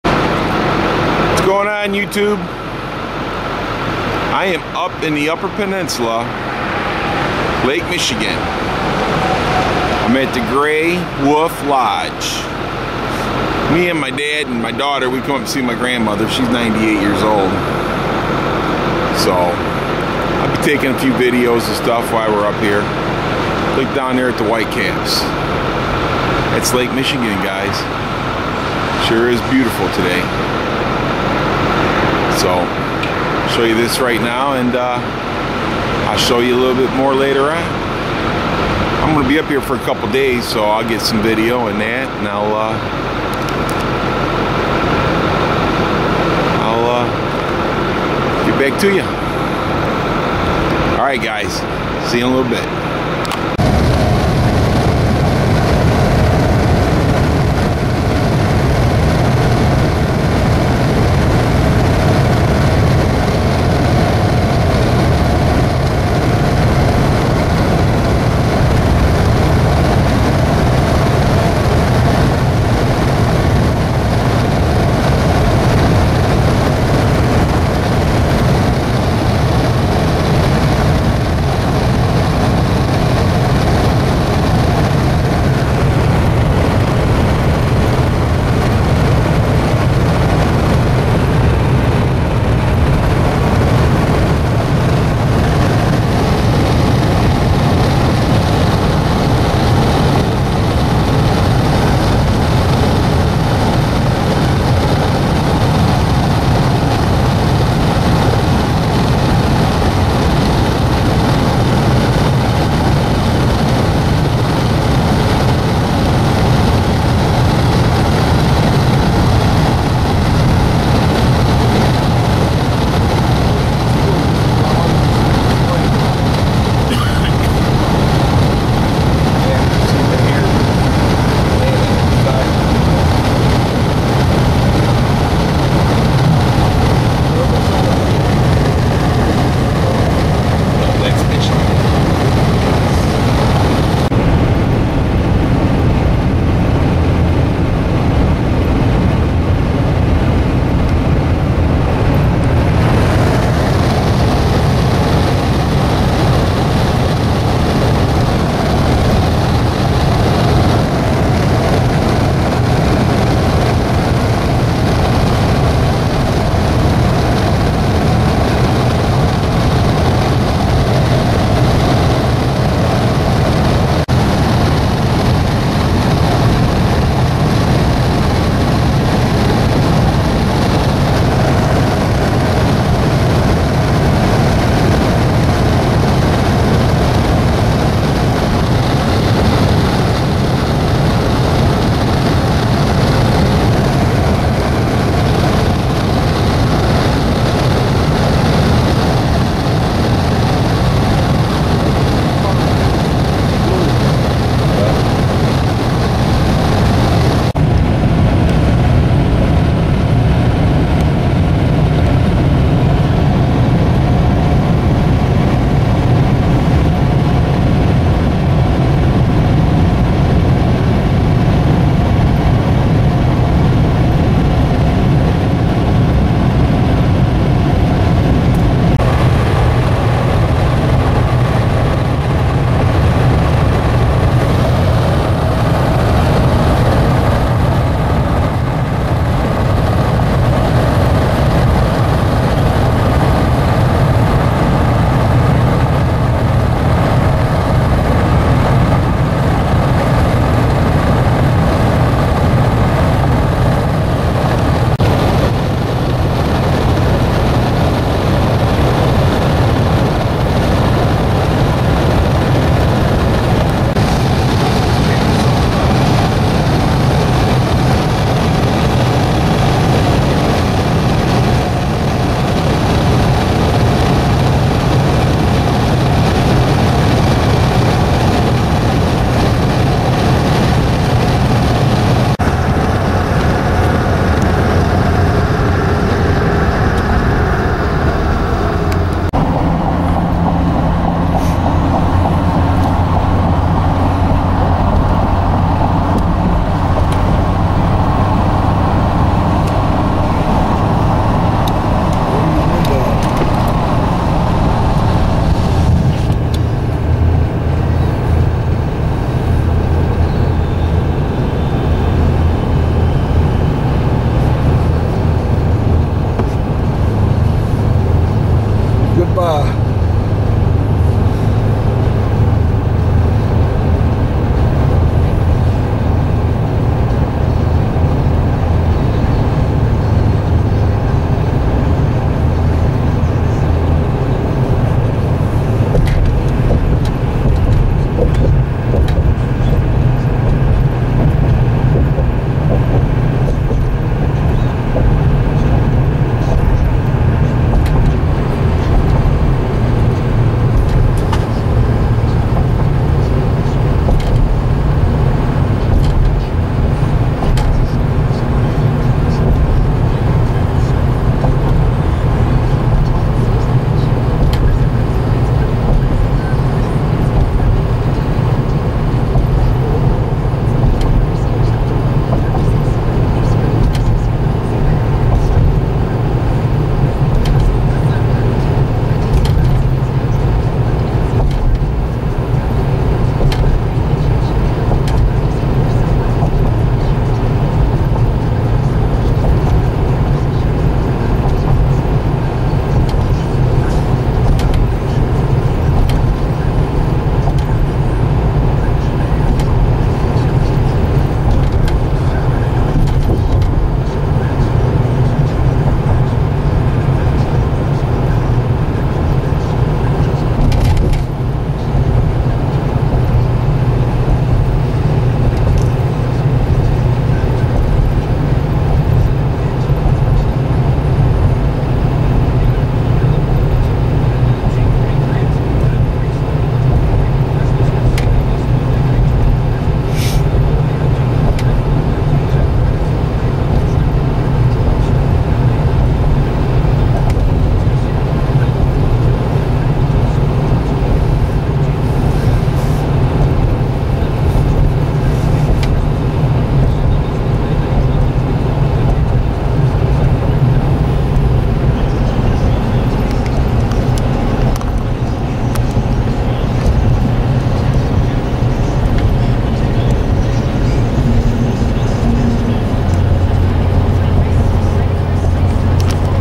In the U P
Just a video when I was in the U.P visiting my 98 year old Grandma. My daughter did the recording. lol